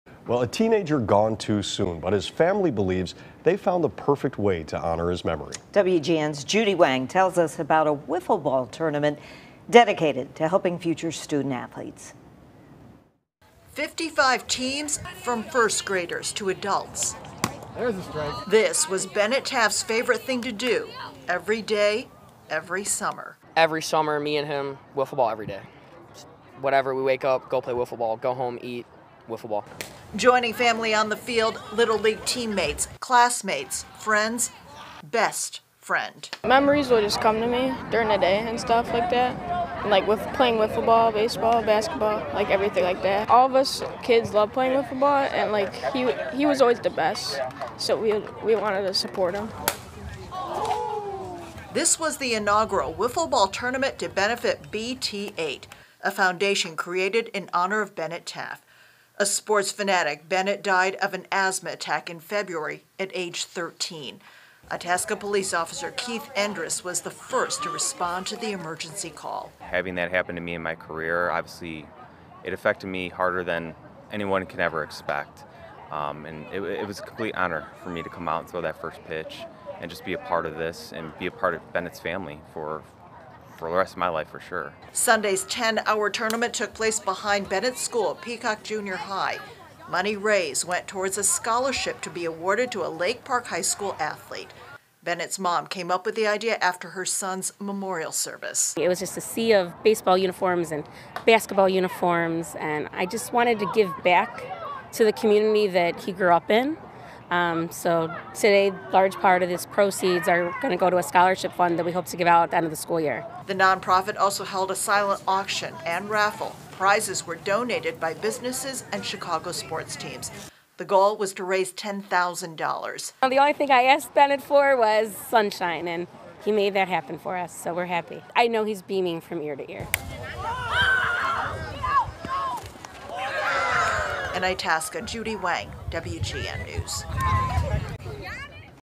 Family, Friends honor life of 13-year old boy with Wiffle ball tournament
A Wiffle ball tournament and silent auction were held on Sunday in honor of a 13-year-old sports fanatic who died of an asthma attack in February.